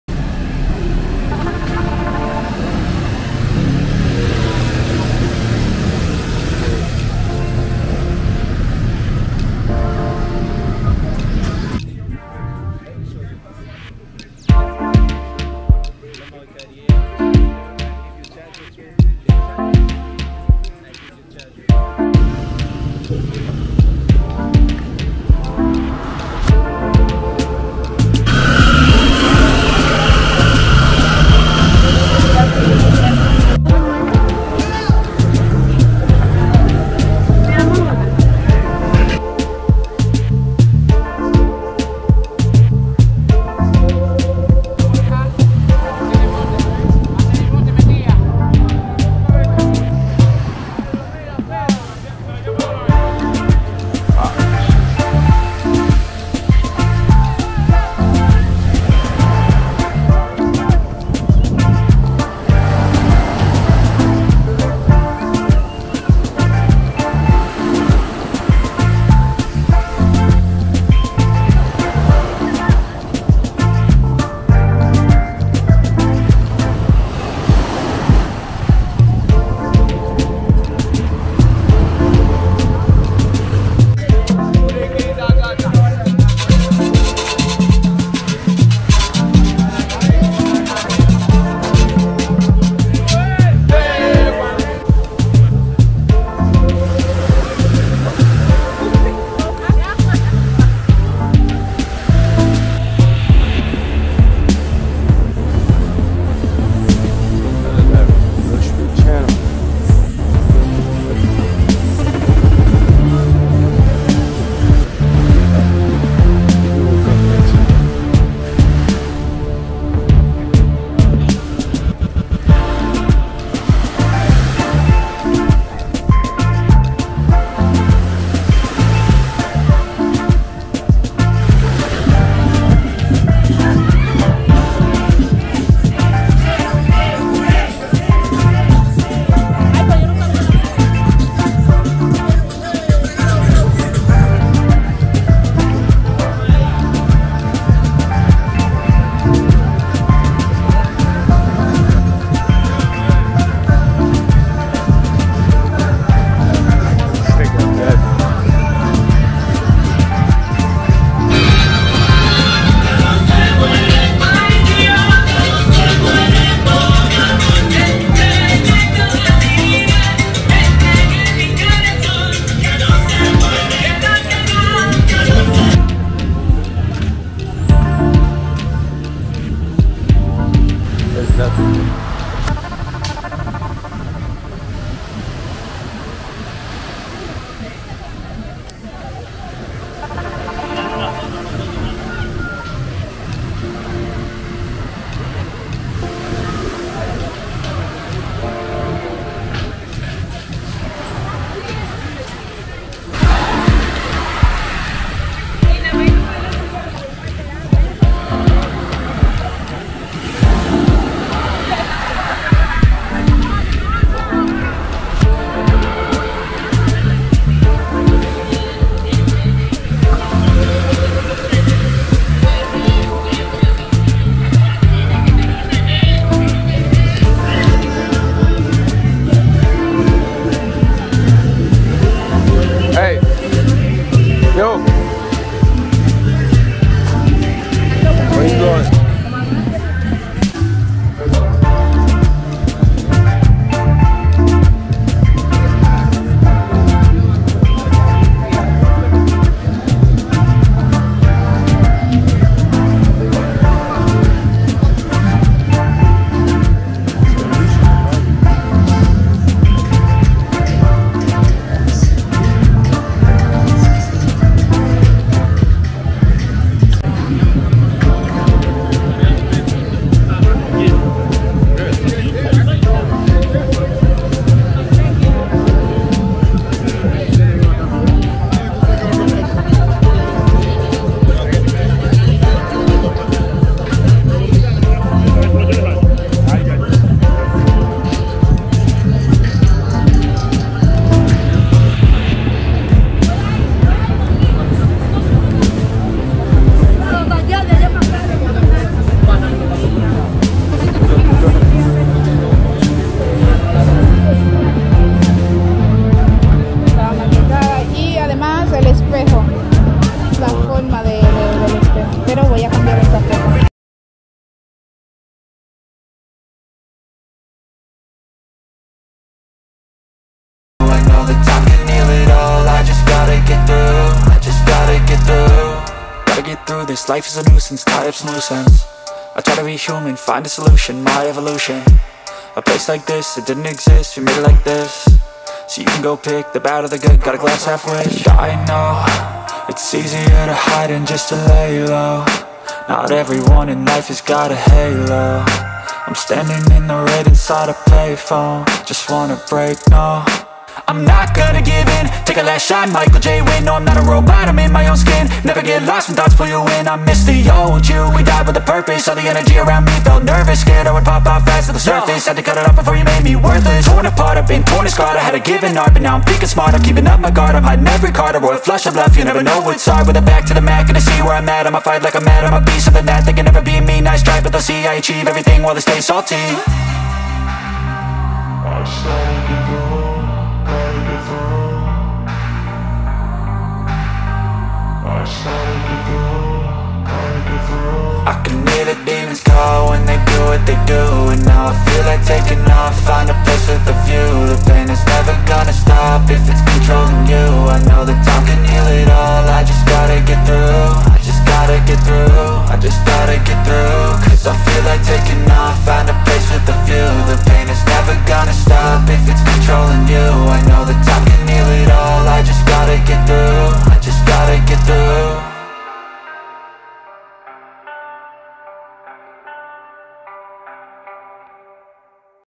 I See Beautiful Women Everywhere in Sosua!
Out in the street of Sosua puerto plata checking out the vibe where i come  across different women walking on the strip and people out there on the beach enjoying themselves in the dominican republic.